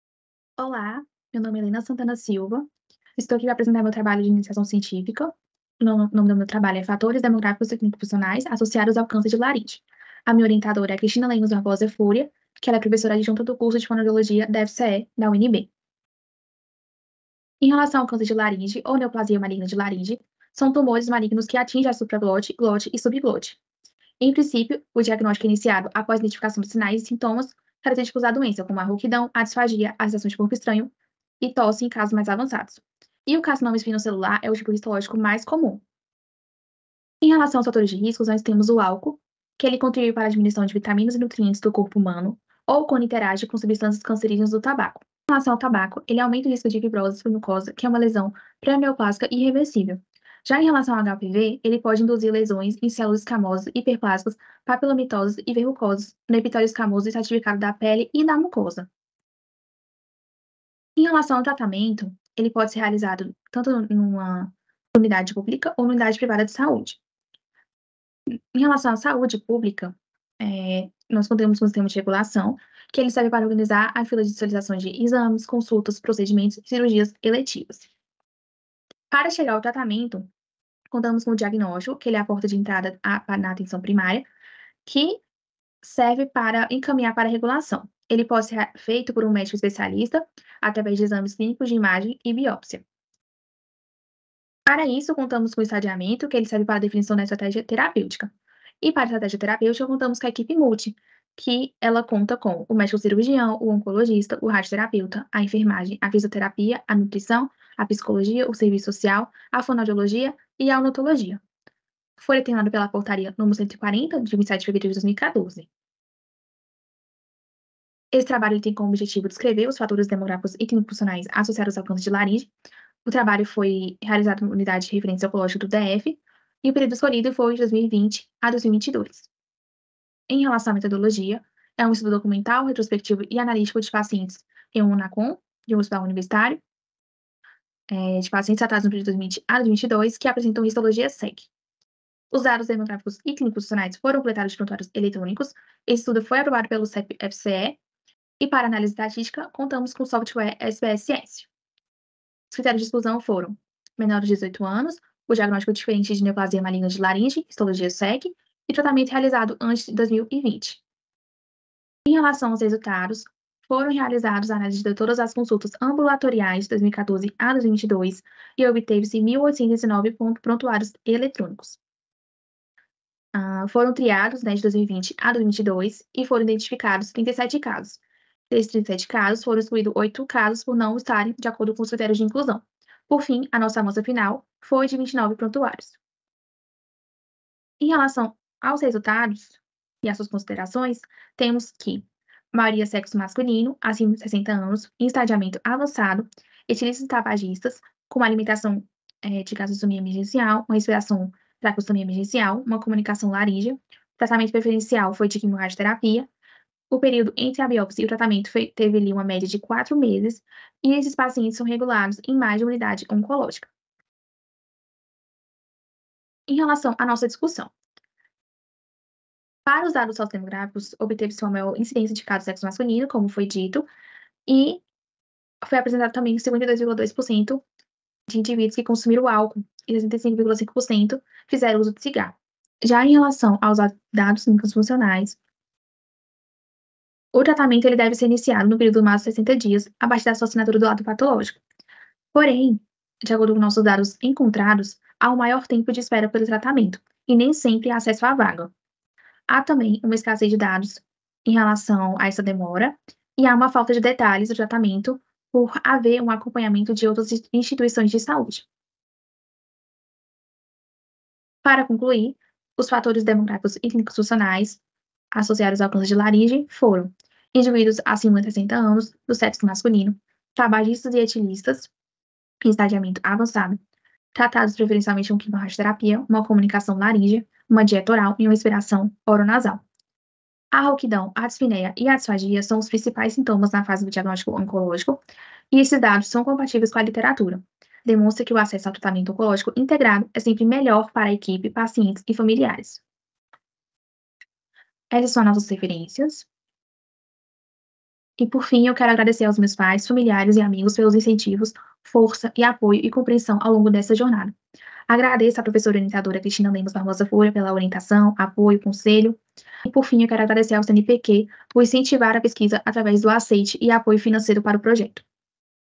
64ec7814f1c350b5b51d5577
Fatores demográficos e clínicos funcionais associados ao câncer de Laringe
PIBIC - UNB: Fatores demográficos e clínicos funcionais associados ao câncer de Laringe (id: 64ec7814f1c350b5b51d5577)

Saúde e Vida - Fonoaudiologia
 As neoplasias de laringe apresentam sinais e sintomas específicos, como rouquidão, disfagia, sensação de corpo estranho e tosse. O carcinoma espinocelular é o tipo histológico mais comum, associado aos fatores de risco, como consumo de álcool, tabaco e ao papilomavírus humano (HPV). O tratamento do câncer é complexo, envolve o trabalho de diversos especialistas para suprir as necessidades do paciente. Sendo os principais objetivos, prolongar a sobrevida e melhorar a qualidade de vida dos pacientes. Podem ser utilizados diferentes métodos cirúrgico parcial ou total, ou clínicos (radioterapia e quimioterapia). O paciente deve ser monitorado por uma equipe multiprofissional. Dessa maneira, o presente estudo tem como objetivo descrever os fatores demográficos e clínicos funcionais associados ao câncer de laringe regulados numa das Unidades de Referência Oncológica do Distrito Federal, como também identificar a presença ou não do tratamento multidisciplinar.

 Estudo documental e retrospectivo. Os dados sócio demográficos (sexo, idade, região, hábitos) e clínicos funcionais (estadiamento, tempo de diagnóstico, tratamento médico e multidisciplicar, comunicação) foram coletados de prontuários eletrônicos de uma Unidade de Alta Complexidade em Oncologia do DF. Sendo assim, as consultas ambulatoriais de cirurgia de cabeça e pescoço da Instituição foram identificadas no sistema de 2014 a setembro de 2022, totalizando num total de 1819 indivíduos, após a retirada das duplicadas. Foi mantido somente os dados descritos em prontuário eletrônico de 2020 a 2022. Os critérios de inclusão foram pacientes com faixa etária acima de 18 anos com diagnóstico de Neoplasia Maligna de Laringe, primária, histologia de Carcinoma Espinocelular (CEC). Para análise estatística, foi utilizado o software Statistical Package for Social Science for Windows (SPSS) e as análises estatísticas descritivas foram realizadas.

 No período de 33 meses foram identificados no sistema interno da instituição, 37 casos de laringe. Foram excluídos 08 por não serem indivíduos com histologia do carcinoma espinocelular ou tratados primariamente, ou ainda pela falta de informações, totalizando 29 prontuários selecionados. Assim, foram identificados indivíduos preferencialmente do sexo masculino (82,8%), acima de 60 anos (69%), em estadios avançados (80%) e consumiram álcool e tabaco associados (48,2%). Quatro pacientes com gastrostomia e traqueostomia emergencial, tempo médio de quatro meses entre a biópsia e tratamento, inicialmente em tratamento de quimioradioterapia, comunicação alaríngea presente em seis indivíduos e em seguimento regular em mais de uma unidade oncológica. Descrições sobre tratamentos multiprofissionais foram somente encontrados de casos atendidos na própria unidade, no qual abrange tratamento médico, enfermagem, odontologia, nutrição, fonoaudiologia, serviço social, psicologia e fisioterapia.

 Em relação aos dados sociodemográficos e clínico funcionais, o câncer de laringe ocorre mais em indivíduos do sexo masculino acima de 60 anos, relacionado ao consumo frequente de álcool e tabaco, sendo a quimioradioterapia o tratamento mais realizado. O ideal é um tratamento integrado que impactará na minimização de sequelas funcionais do indivíduo e contribuirá para a rotatividade e diminuição da quantidade de pacientes. Entretanto, não ocorre na prática e pode ser atribuído a profissionais insuficientes e escassez de instalações, o que corrobora para que os atendimentos de pacientes com câncer de cabeça e pescoço sejam realizados de maneira fragmentada. Não foi possível mensurar dados a respeito do período entre os sintomas e o tratamento, local de realização do tratamento cirúrgico, radioterápico, quimioterápico e acompanhamento com a equipe multidisciplinar. Nesse ínterim, os fatores sociodemográficos e clínicos funcionais descritos são compatíveis com a literatura.